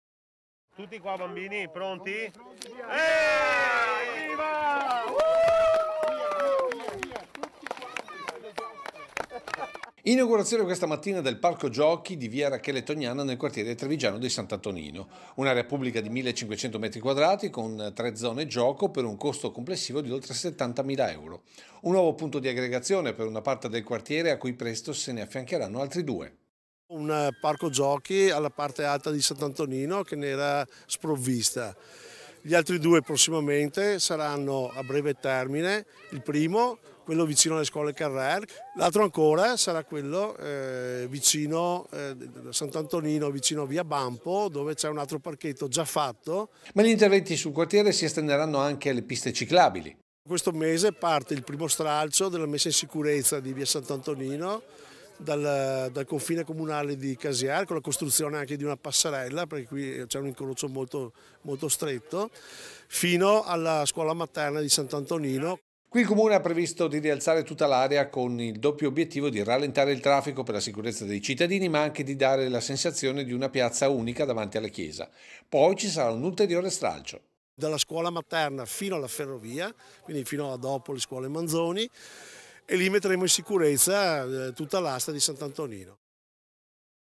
NUOVI PARCHI GIOCHI E NUOVE PISTE CICLABILI PER S.ANTONINO | 04/04/2022
A3 NEWS Treviso 04/04/2022 - TREVISO - Apertura questa mattina nel quartiere trevigiano di S.Antonino di un nuovo parco giochi. Dall’assessore Zampese anche le novita’ per la rete di piste ciclabili.   || Inaugurazione questa mattina del parco giochi di via Rachele Tognana nel quartiere trevigiano di S.Antonino. Un’area pubblica di 1500 metri quadrati con tre zone gioco per un costo complessivo di oltre 70 mila euro. Un nuovo punto di aggregazione per una parte del quartiere a cui presto se ne affiancheranno altri due.Ma gli interventi sul quartiere si estenderanno anche alle piste ciclabili.Qui il comune ha previsto di rialzare tutta l’area con il doppio obbiettivo di rallentare il traffico per la sicurezza dei cittadini ma anche di dare la sensazione di una piazza unica davanti alla chiesa. Poi ci sara’ unulteriore stralcio. - Intervistati SANDRO ZAMPESE (Assessore lavori pubblici comune di Treviso) (Servizio di Lucio Zanato) - Segui Antenna Tre anche sul digitale terrestre! Visita il sito